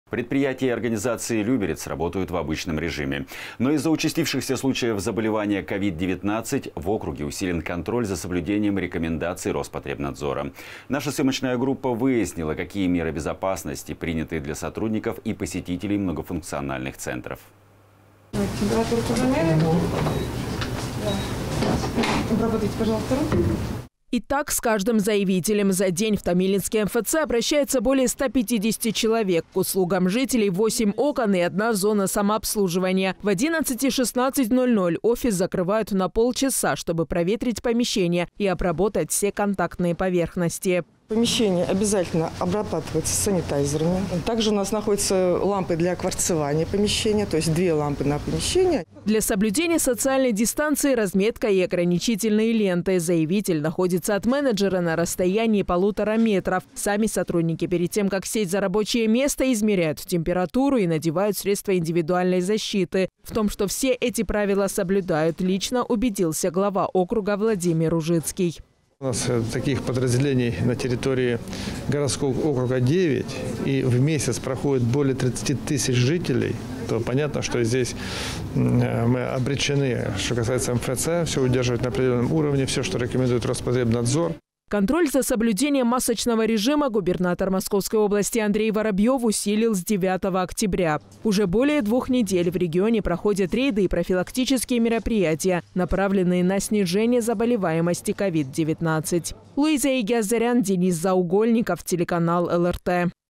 МФЦ во время пандемии: правила приема и распорядок дня
Предприятия и организации Люберец работают в обычном режиме. Но из-за участившихся случаев заболевания COVID-19, в округе усилен контроль за соблюдением рекомендаций Роспотребнадзора. Наша съемочная группа выяснила, какие меры безопасности предприняты для сотрудников и посетителей многофункциональных центров.

"Давайте температуру замерим, обработайте, руки".

И так с каждым заявителем. За день в томилинский МФЦ обращается более 150 человек. К услугам жителей 8 окон и одна зона самообслуживания. В 11 и 16:00 офис закрывают на полчаса, чтобы проветрить помещения и обработать все контактные поверхности.
СНХ 1188 ТИТР: Марина Тахбеева, начальник отдела «Томилинский» МФЦ г.о. Люберцы
02.30 Помещения обязательно обрабатываются санитайзерами 02.33 СКЛ  02.35 Также у нас находятся лампы для кварцевания помещения. Две лампы на помещение  02.39
ЗК: Для соблюдения социальной дистанции – разметка и ограничительные ленты. Заявитель находится от менеджера на расстоянии полутора метров. Сами сотрудники, перед тем, как сесть за рабочее место, измеряют температуру и надевают средства индивидуальной защиты. В том, что все эти правила соблюдают, лично убедился глава округа Владимир Ружицкий.

Владимир Ружицкий, глава г.о. Люберцы:
"У нас таких подразделений на территории городского округа – 9, в месяц проходит более 30 тысяч жителей. Понятно, что мы обречены, все, что касается МФЦ, все удерживать на определенном уровне, все, что рекомендует Роспотребнадзор".

Контроль за соблюдением масочного режима губернатор Московской области Андрей Воробьев усилил с 9 октября. Уже более двух недель в регионе проходят рейды и профилактические мероприятия, направленные на снижение заболеваемости COVID-19.